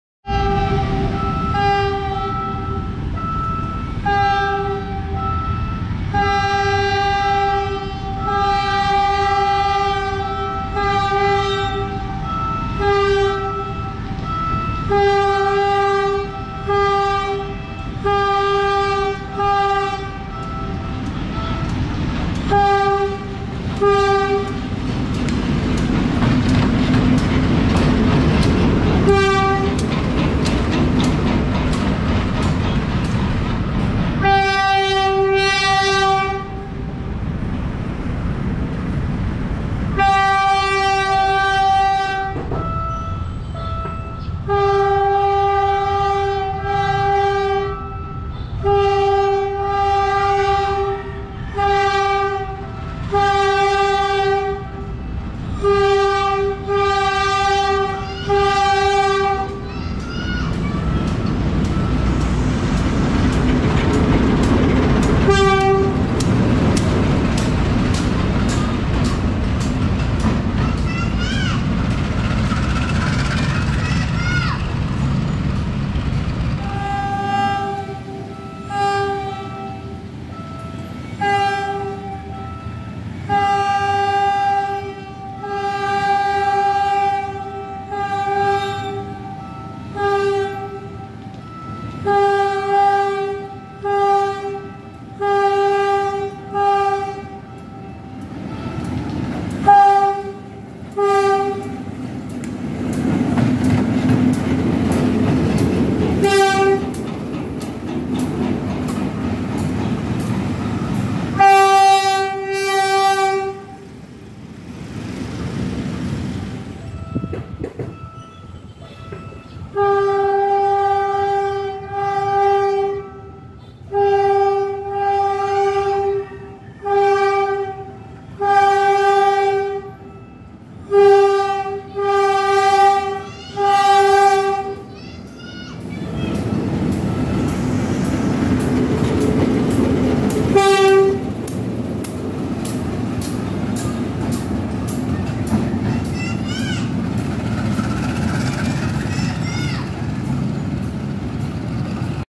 PNR DEL 921 Turnaround at Tayuman Wye
Taken June 10th 2023

DEL 921 from Shorthood Southbound to Shorthood Northbound maneuvered at Tayuman Wye